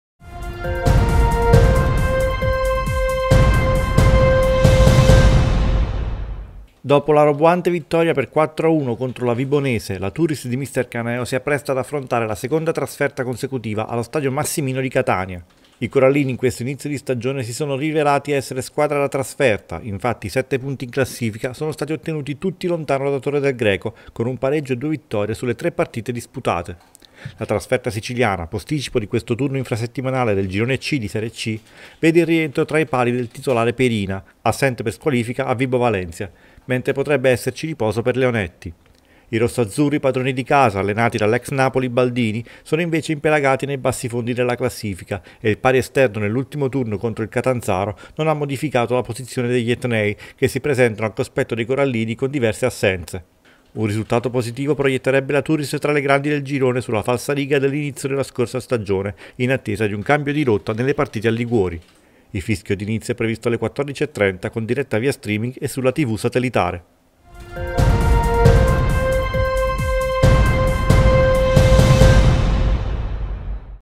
Catania-Turris: i corallini in cerca di conferme
Dopo la roboante vittoria per 4-1 contro la Vibonese, la Turris di mister Caneo si appresta ad affrontare la seconda trasferta consecutiva allo stadio “Massimino” di Catania.
I corallini in questo inizio di stagione si sono rivelati essere squadra da trasferta, infatti i 7 punti in classifica sono stati ottenuti tutti lontano da Torre del Greco con 1 pareggio e 2 vittorie sulle 3 partite disputate.
La trasferta siciliana, posticipo di questo turno infrasettimanale del girone C di serie C, vede il rientro tra i pali del titolare Perina, assente per squalifica a Vibo Valentia, mentre potrebbe esserci riposo per Leonetti.
I rossoazzurri padroni di casa, allenati dall’ex Napoli Baldini, sono invece impelagati nei bassifondi della classifica e il pari esterno nell’ultimo turno contro il Catanzaro non ha modificato la posizione degli etnei che si presentano al cospetto dei corallini con diverse assenze.
Un risultato positivo proietterebbe la Turris tra le grandi del girone sulla falsariga dell’inizio della scorsa stagione in attesa di un cambio di rotta nelle partite al Liguori.
Il fischio d’inizio è previsto alle 14.30 con diretta via streaming e sulla TV satellitare.